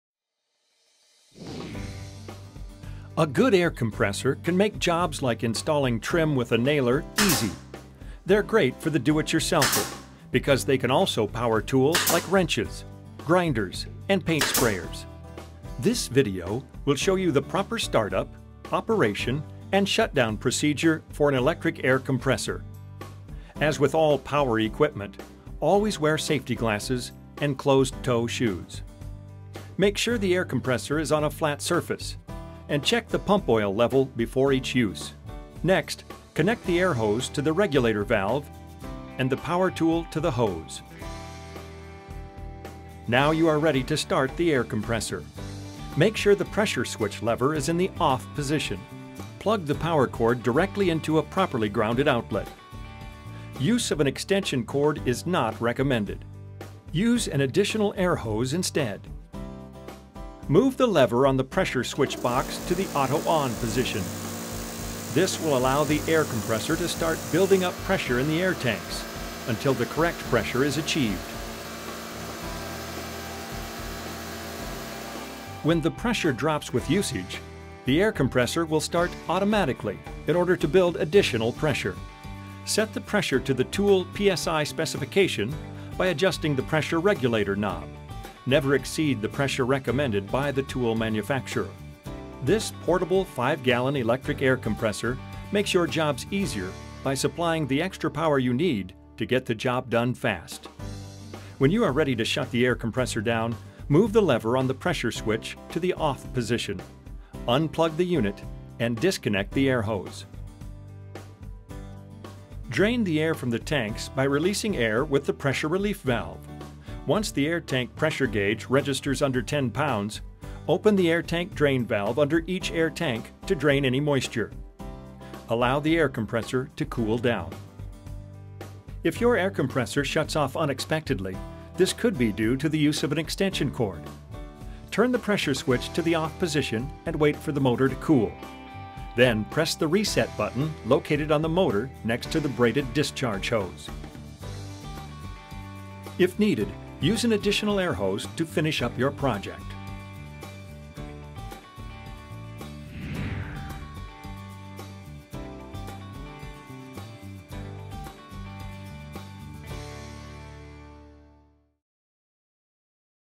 How To Set Up An Electric Air Compressor
This video will show you the proper start-up, operation, and shutdown for an industrial electric air compressor. The product used is a  Mi-T-M 5 Gallon Electric Air Compressor.

Who is Mi-T-M?
Mi-T-M Corporation is a leading designer and manufacturer of high-quality industrial equipment including pressure washers, air compressors, and generators.